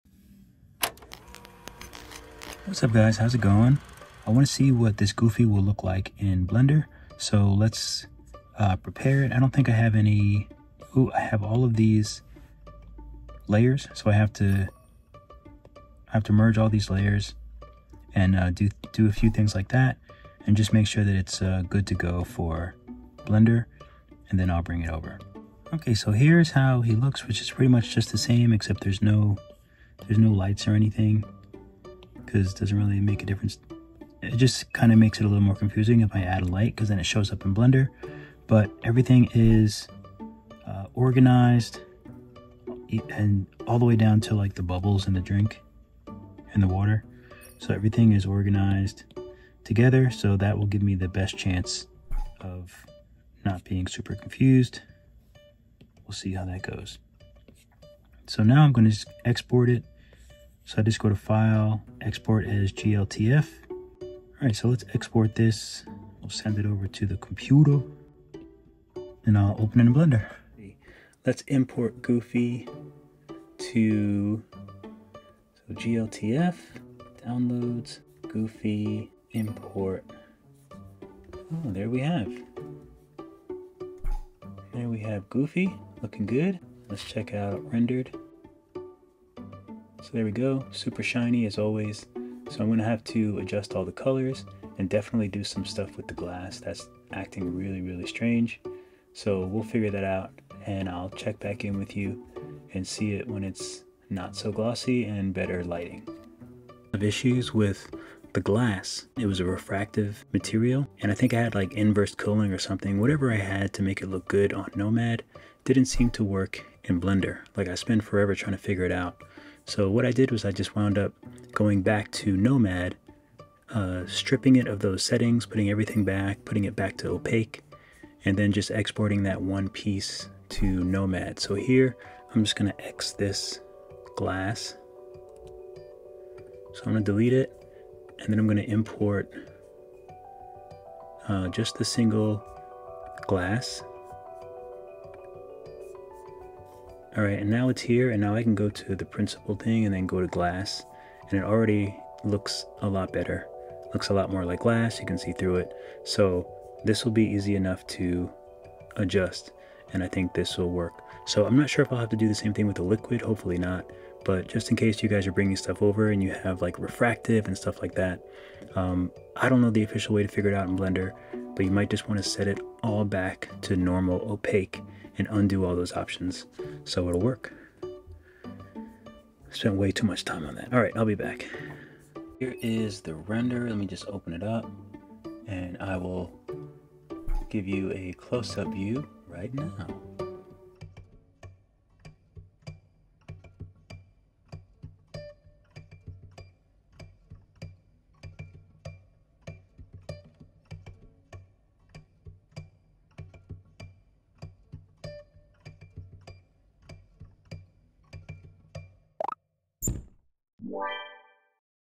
Nomad Sculpt Render Exported to Blender Cycles | Sleepy Goofy!
Sleepy Goofy rendered in Blender! This one may be fun to 3D print too. Enjoy!